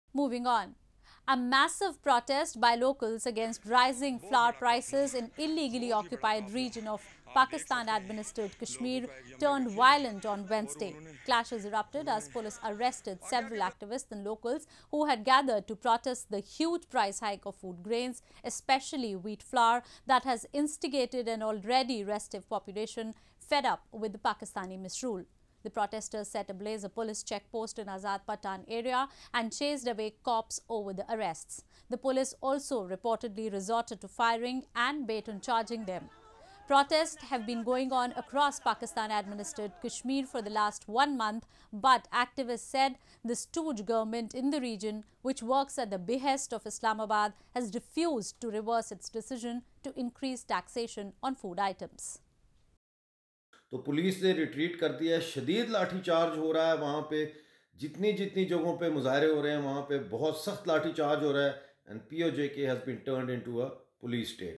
Anti-Pakistan protest turns violent in Pakistan-administered Kashmir; police atrocities continue
A massive protest by locals against rising flour prices in illegally-occupied region of Pakistan-administered Kashmir turned violent on Wednesday.